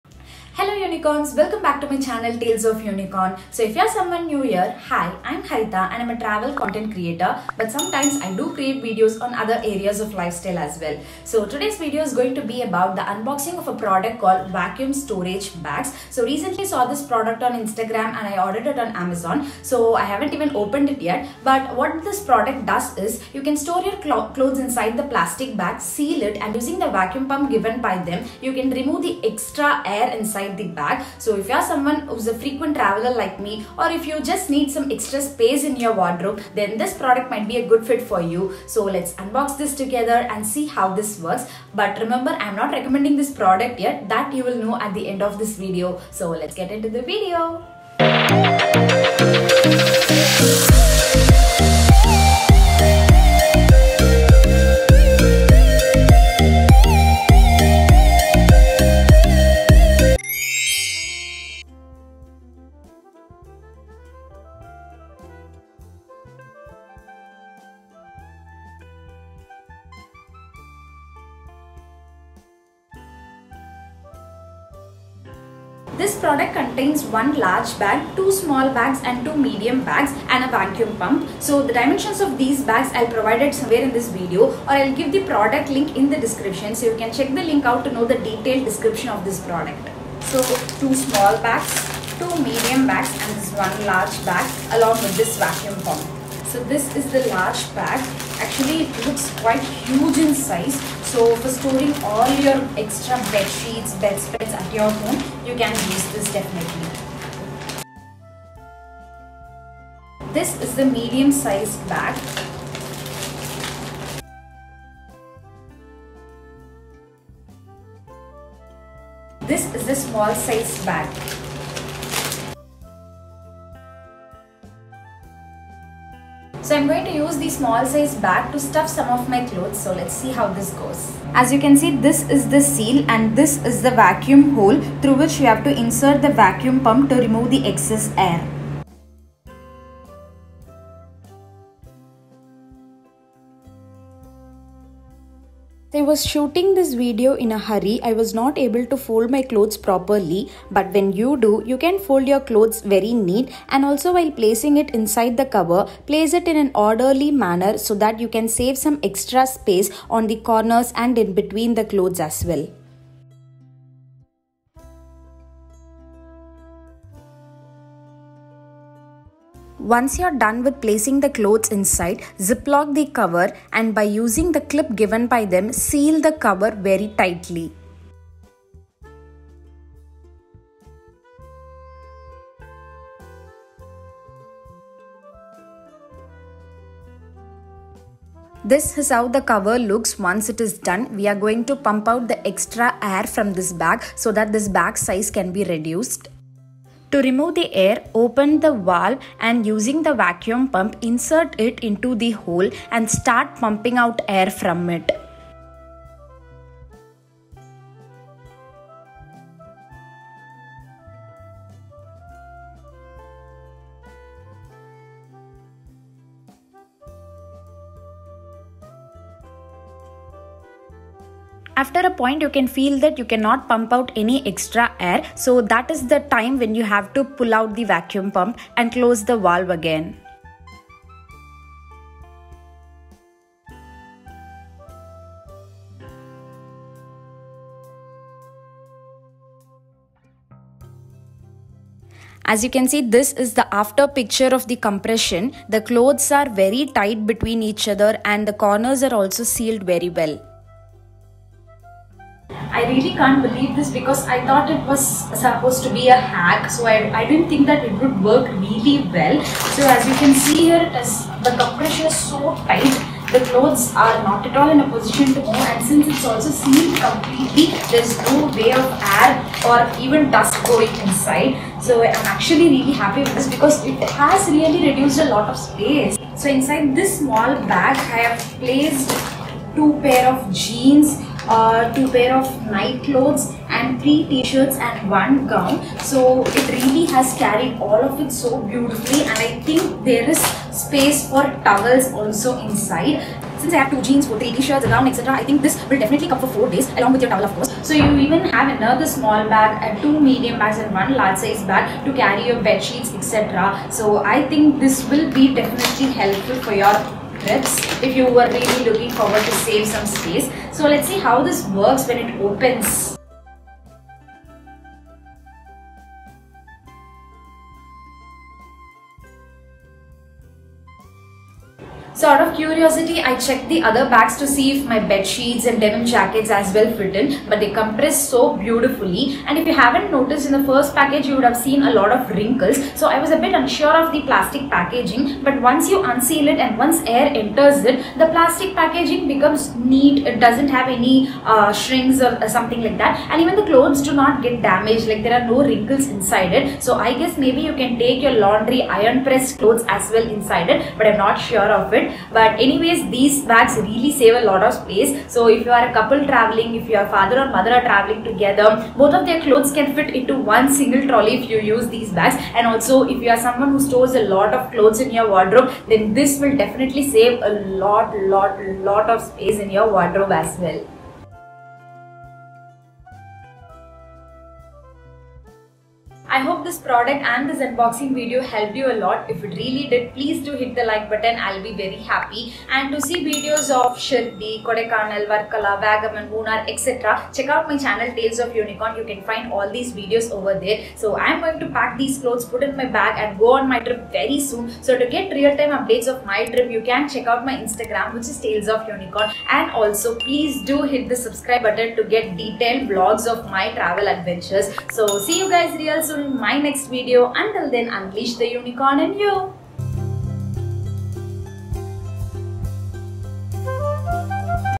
Travel Hack🤯to save SPACE | VACUUM BAGS 🥳 | Wardrobe Space Saver 💯| Tales of Unicron|Travel tips
Hello Unicorns.!!! In this video, you can see the unboxing and review of Vacuum storage bags that literally blew my mind.!
It helps in saving a lot of storage space in your backpack as well as your wardrobe.

If this product is not deliverable to your pincode, try searching for Vacuum storage bags and you can purchase from anything that pops up and has good reviews.